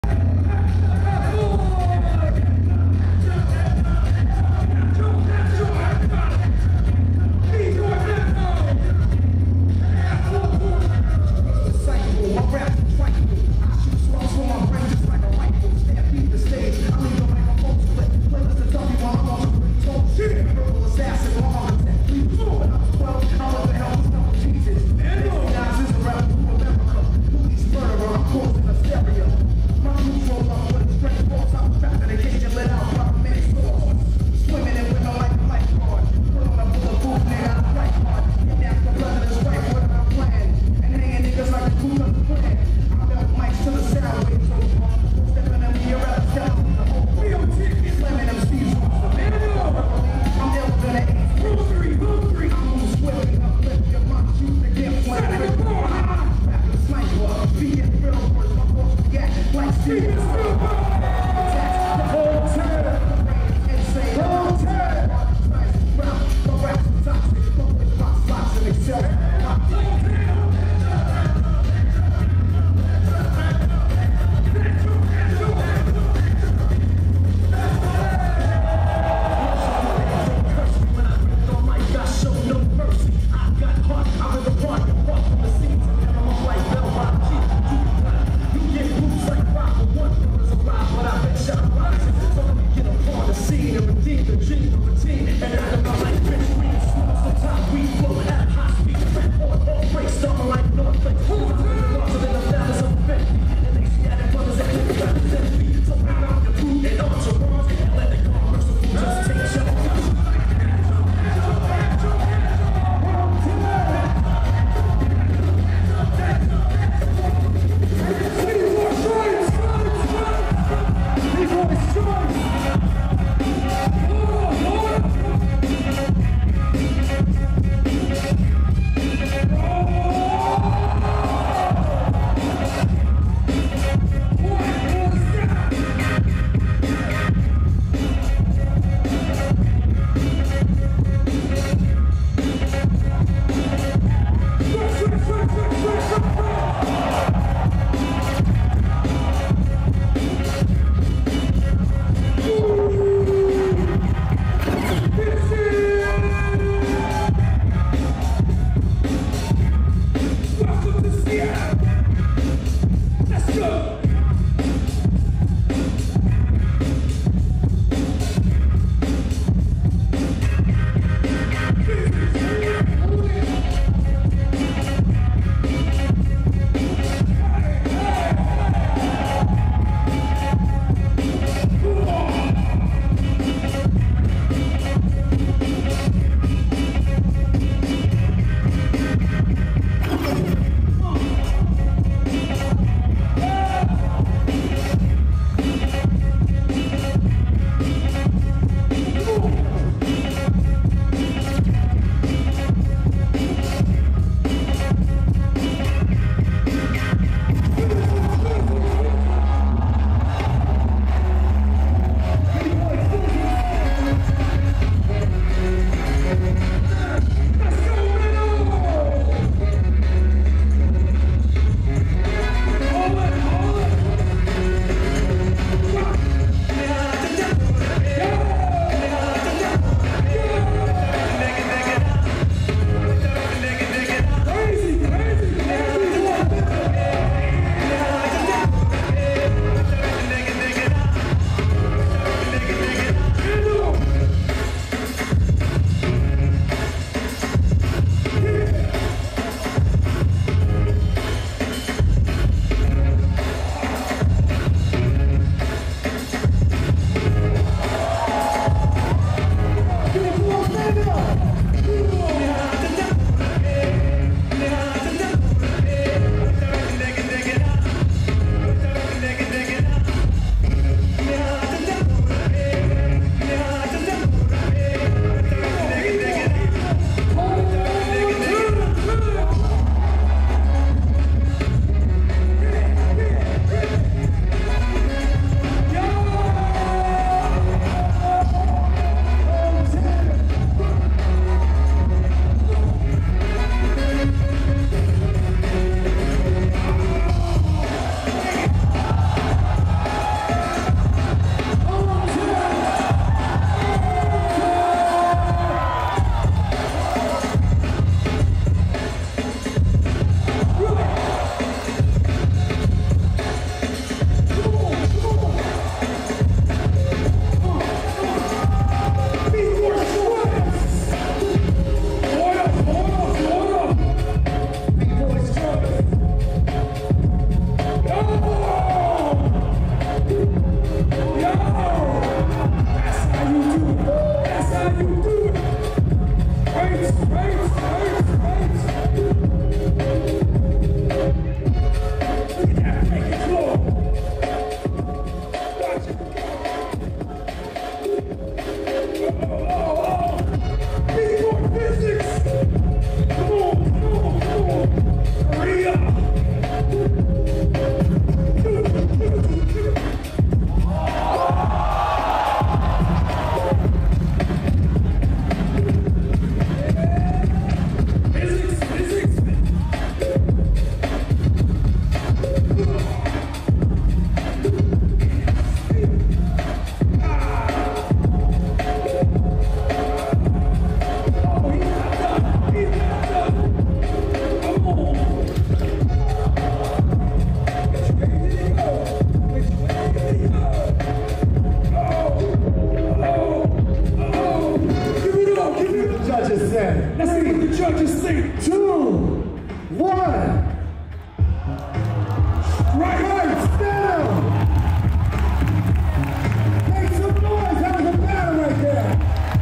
Hong 10/Physicx (Project Soul) Vs Menno/Stripes -Top 16 - Red Bull Lords of The Floor
Red Bull Lords of The Floor (2024)
"Breaking Since 2001 "

"After more than two decades, Red Bull Lords Of The Floor is back. The global 2v2 crew battle returns to celebrate the history of breaking while also spotlighting the future of the community.
Red Bull Lords Of The Floor will feature 16 international duos who are either looking to take care of unfinished business or make their mark on the global stage. 14 crews have been invited, 2 crew spots is available for you to battle."

Top 16

Winners
Menno/Stripes

Judges
AT
The End  (Gamblerz)
Storm (Battle Squad)
Orb (Circle Of Fire)
Beta (Heartbreakerz)

DJ
Fleg
Mar Ski

Emcee/Host
Jo Koy
Ivan The Urban Action Figure